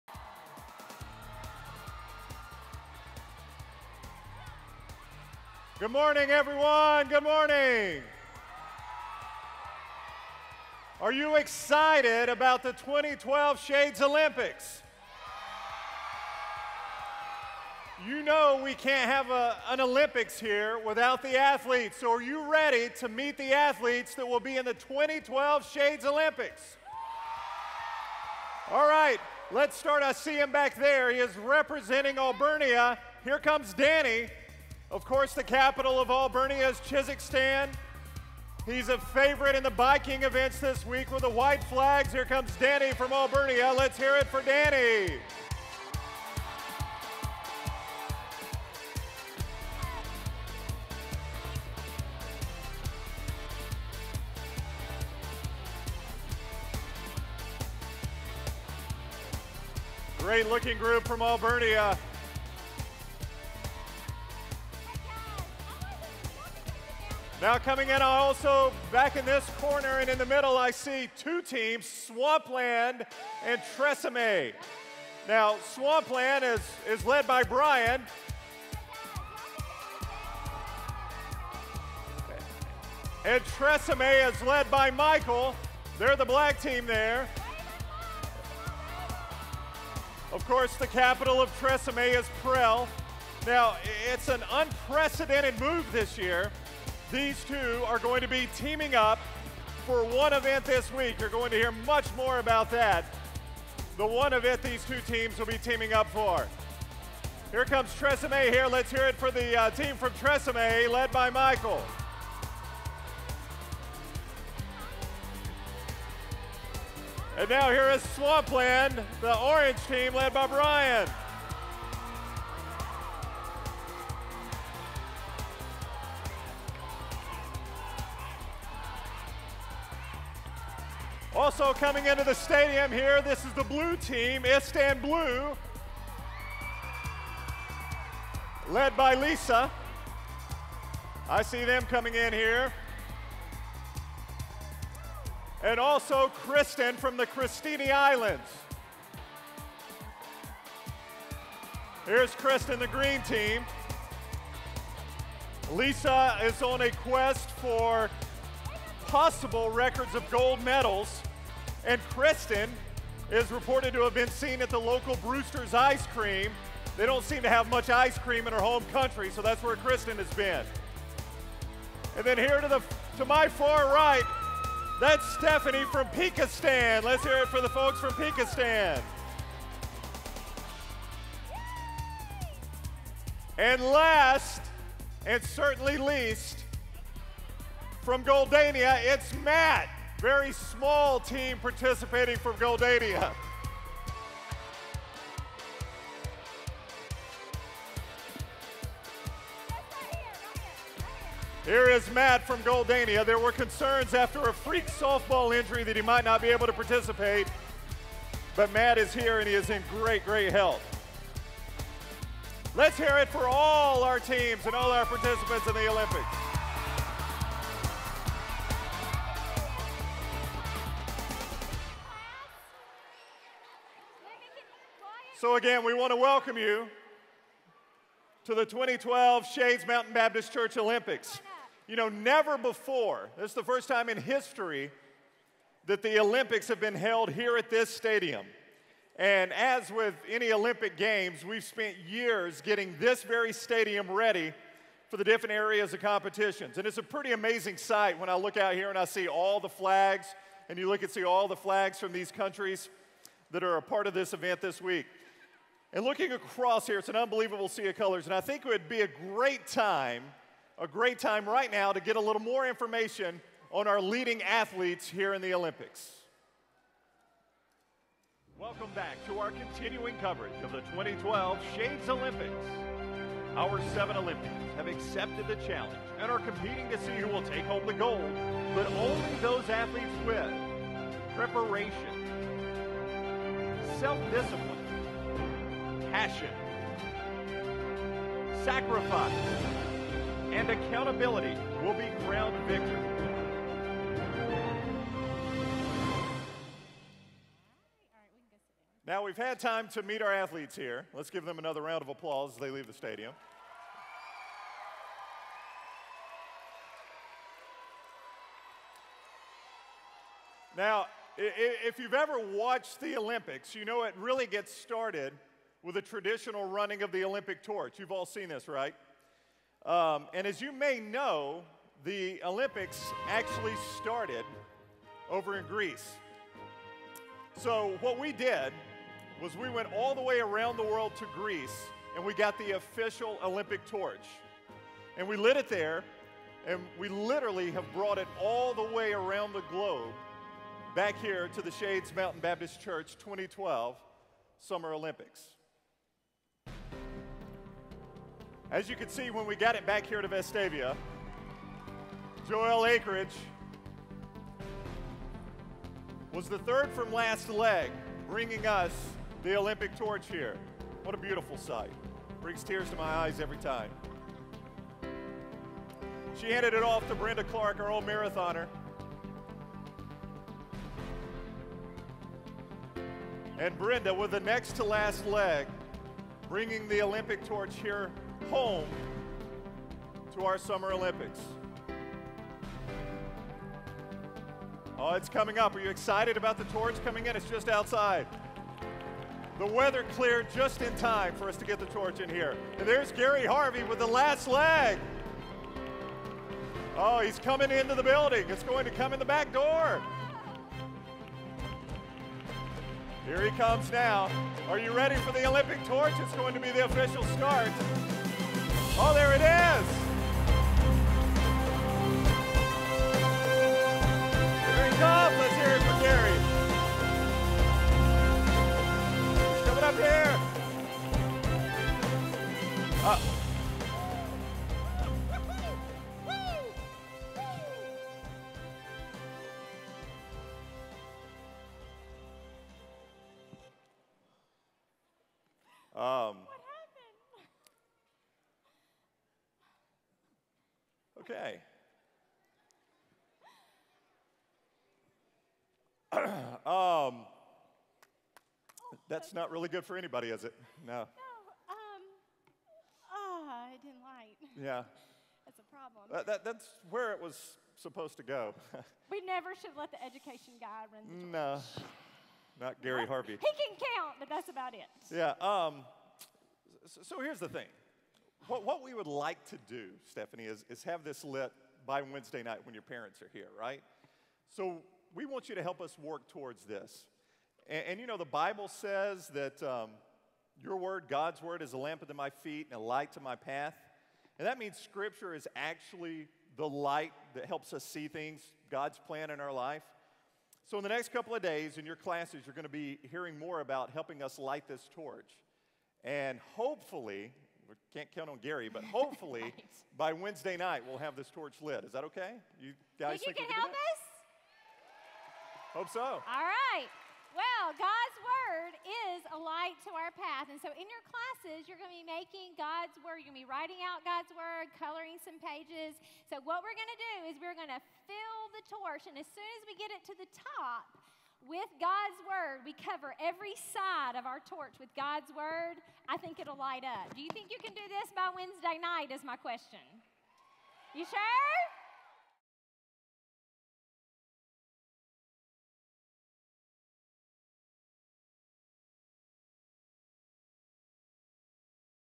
VBS2012: Accept the Challenge
First day of Vacation Bible School at Shades Mountain Baptist Church in Birmingham, AL